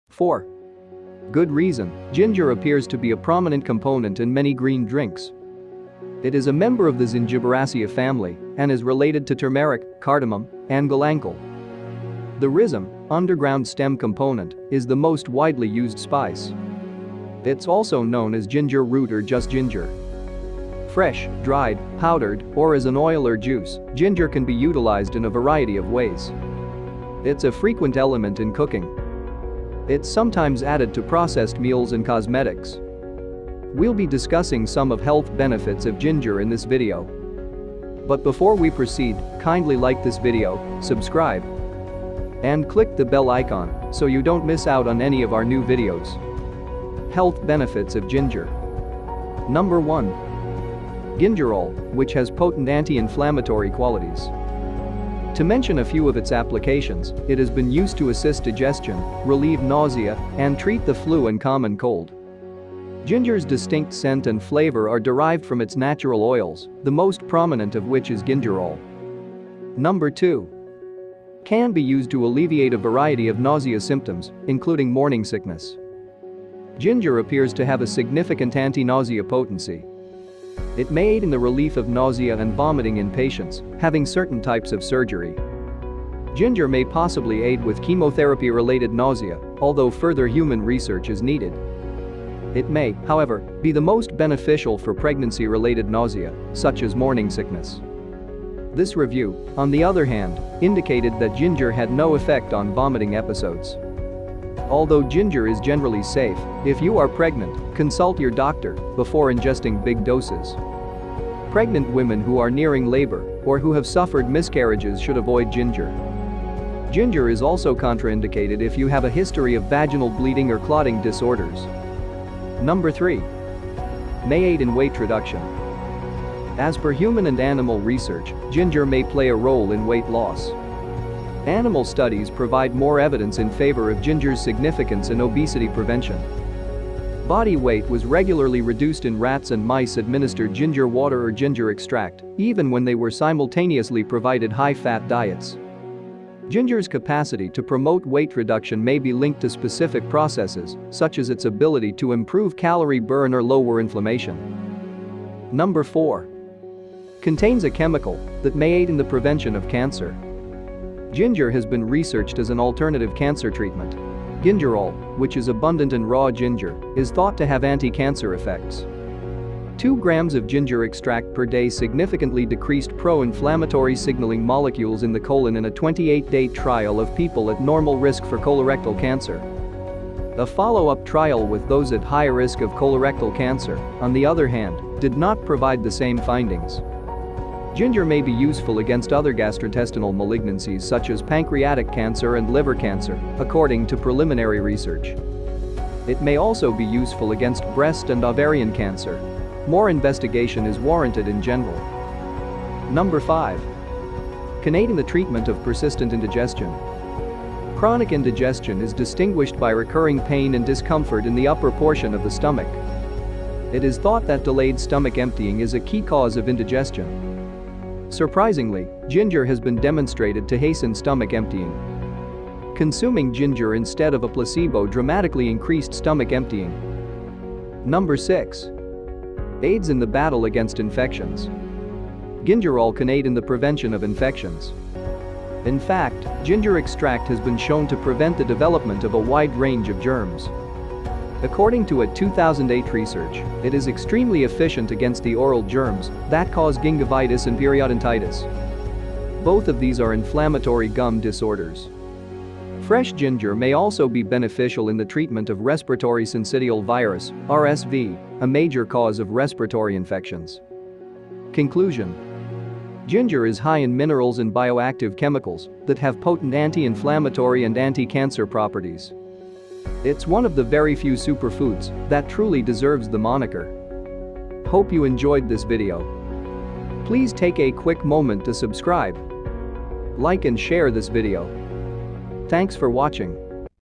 Amazing Health benefits of ginger
For good reason, ginger appears to be a prominent component in many green drinks. It is a member of the Zingiberaceae family and is related to turmeric, cardamom, and galangal.

The rhizome (underground stem component) is the most widely used spice. It's also known as ginger root or just ginger.

Fresh, dried, powdered, or as an oil or juice, ginger can be utilized in a variety of ways. It's a frequent element in cooking. It’s sometimes added to processed meals and cosmetics.